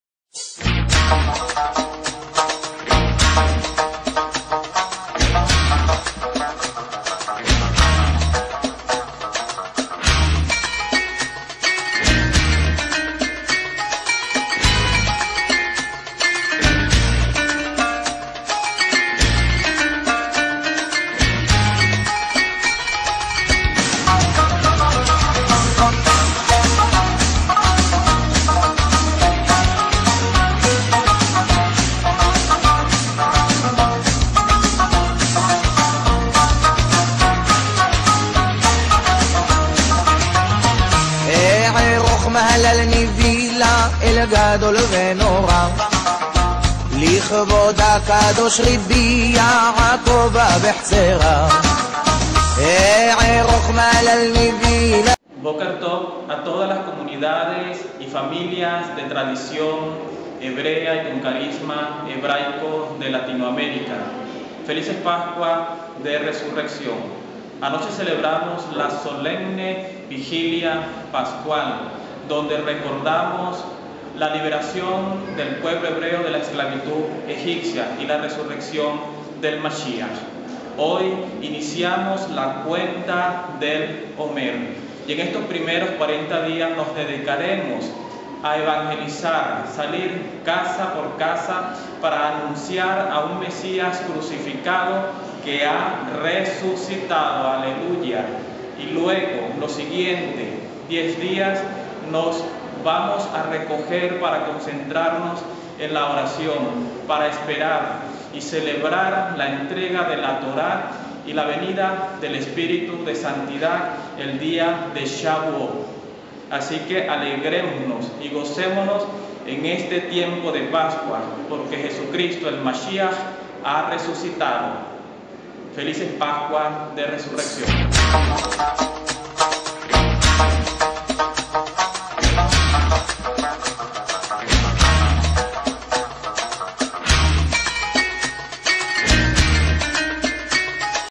PALABRAS DEL PBRO.: AARON COHEN: MASHIAJ HU KAM!! CRISTO RESUCITÓ. HEBREOS CATOLICOS.
MASHIAJ HU KAM!! BE'EMET HU KAM!!!
CRISTO RESUCITO!!!  VERDADERAMENTE RESUCITO!!!